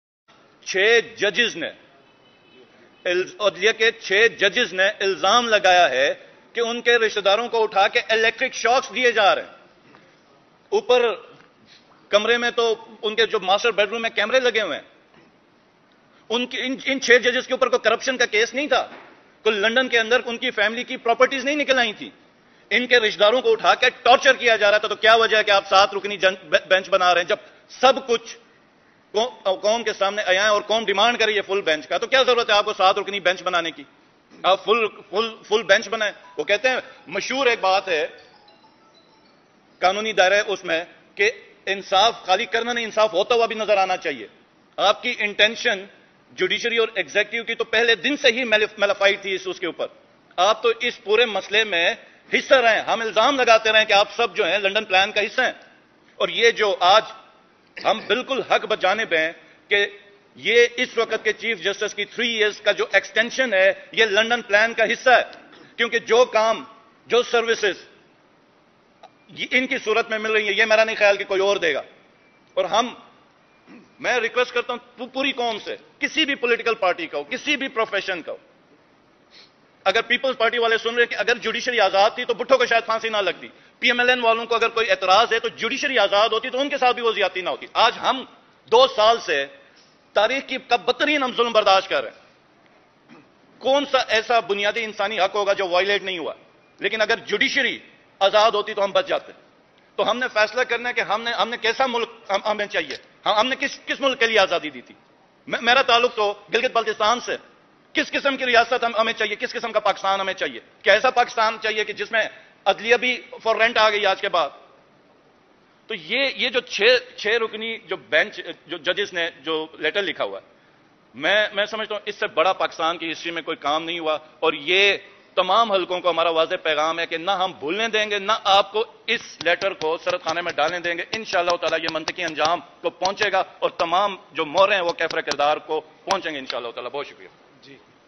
ان ججز کے لیے فل کورٹ بنانے میں کیا رکاوٹ ہے سابق وزیراعلی گلگت بلتستان خالد خورشید
News & Vlog Channel Please Follow Our Facebook